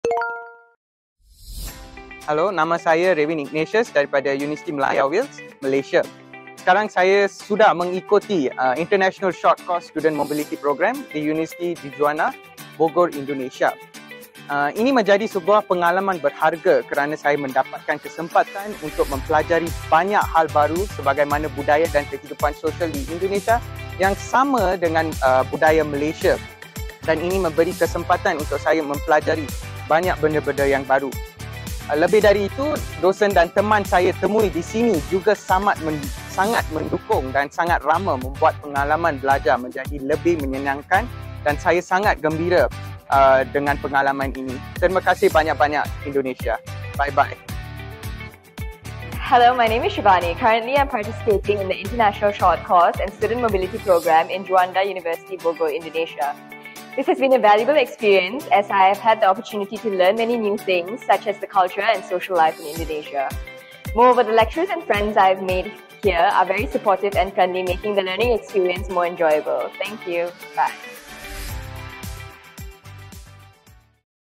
Testimoni Mahasiswa Internasional
Assalamu’alaikum wr.wb.

Sudah lihat cuplikan keseruan rangkaian Program International Short Course dan Student Mobility kemarin kan? 🤩

Apa sih kata mereka setelah mengikuti program ini? Yuk liat lagi videonya! 😇